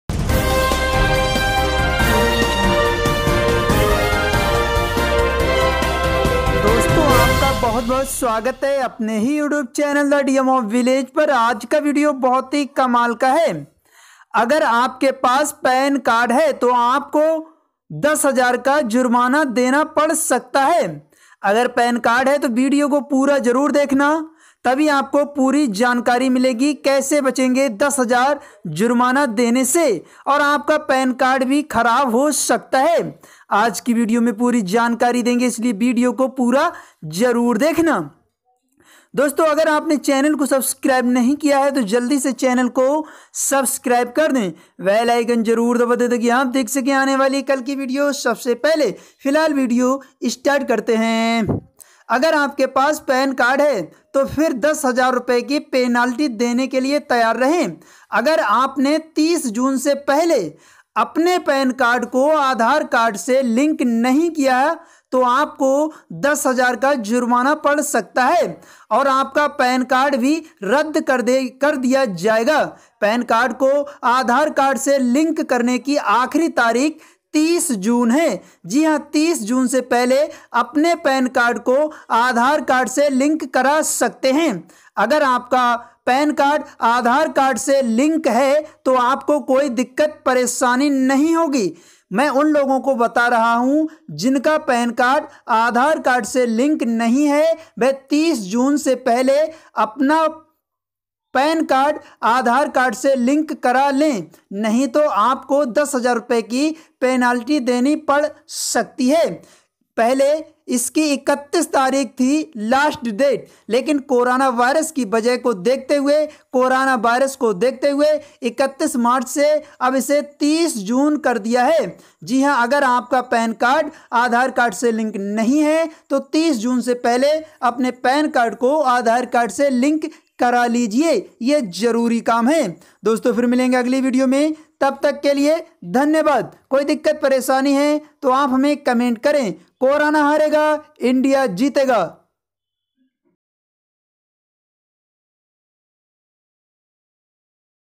30 जून से पहले पैन कार्ड वाले यह काम जरूर करा लें नहीं तो पैन कार्ड रद्द
दोस्तों आज की वीडियो में हमने जानकारी दी है अगर आपके पास पैन कार्ड है तो अपने पैन कार्ड को आधार कार्ड से 30 जून से पहले जरूर अपडेट करा लीजिए पैन कार्ड को आधार कार्ड से लिंक कराने की आखिरी तारीख सरकार ने 30 जून 2020 कर दी है पहले आखिरी तारीख 31 मार्च 2020 थी लॉक डाउन की वजह से अब 30 जून कर दिया है जानकारी अच्छी लगे तो लाइक करो शेयर करो पैन कार्ड उन्हीं लोगों का आधार कार्ड से लिंक होगा जिन्होंने अपना पैन कार्ड आधार कार्ड से लिंक नहीं कराया है